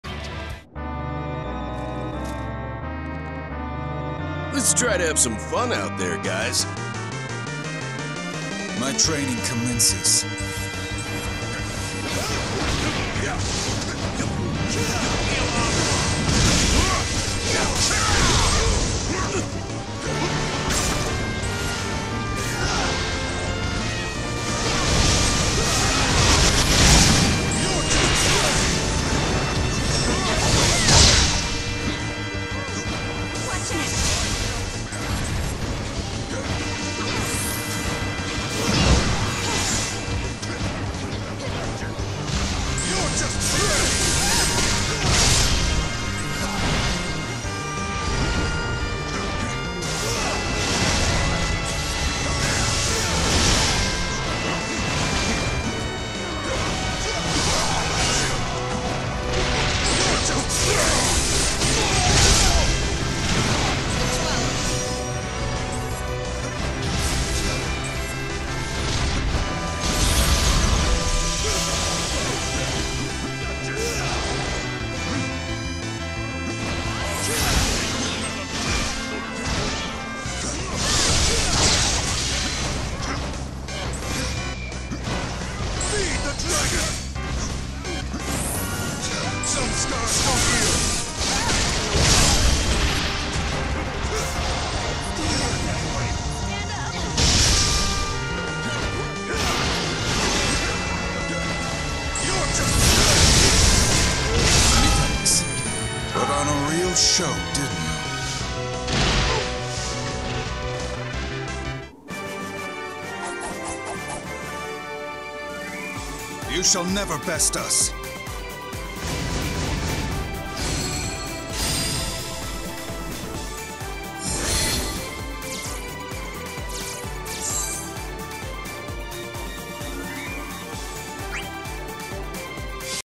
Never Leave Kain Alone
The smartest move the enemy team ever makes happens within the first 10 seconds of the match and nothing comes even remotely close to that for the remainder.